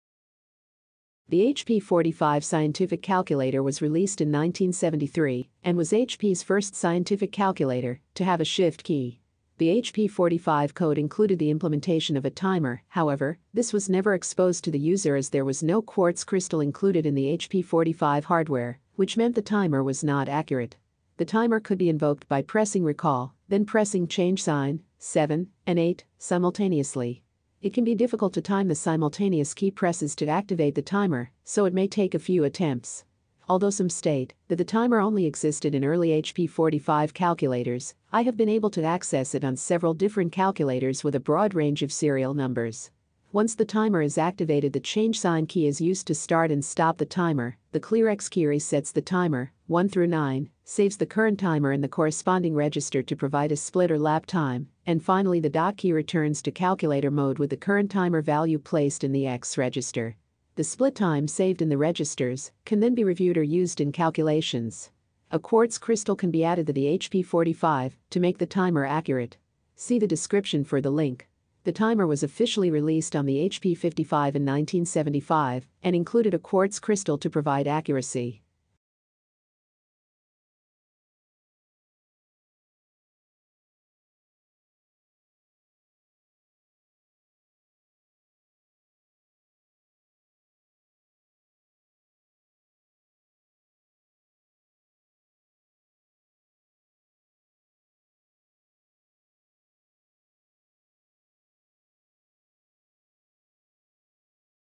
HP 45 Scientific Calculator Hidden Timer
Demonstration of the timer included in the HP 45 code but not officially released or exposed to the user as there was no quartz crystal included in the hardware to make it accurate.

To invoke the timer press and release RCL and then press CHS, 7 and 8 simultaneously. It might take a few goes to activate it.

While in the timer the following keys are used:

CHS - start or stop the timer
1-9 - store current timer value in corresponding register
CLX - reset the timer
dot - exit timer and save current timer value in X
ENTER - exit timer without saving timer

See the following for how to add a quartz crystal to the HP 45 to make the timer accurate